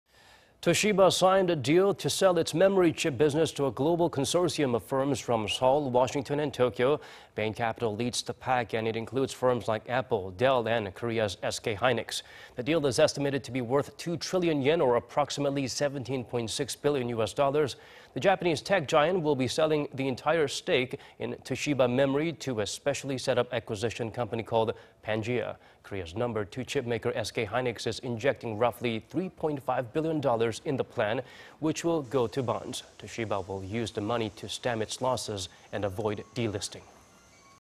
Toshiba to sell memory chip unit in deal worth US$17.6 bil.
"도시바, SK 등 한미일연합과 반도체사업 매각계약 체결"

  Toshiba signed a deal to sell its memory chip business to a global consortium of firms from Seoul, Washington and Tokyo.
  Bain Capital leads the pack... and it includes firms like Apple, Dell... and Korea's SK Hynix.
The deal is estimated to be worth 2-trillion yen... or approximately 17-point-6 billion U.S. dollars.
The Japanese tech giant will be selling its entire stake in Toshiba Memory to a specially set-up acquisition company called Pangea.
  Korea's number 2 chipmaker SK Hynix is injecting roughly 3-point-5 billion dollars in the plan, which will go to bonds... Toshiba will use the money to stem its losses and avoid delisting.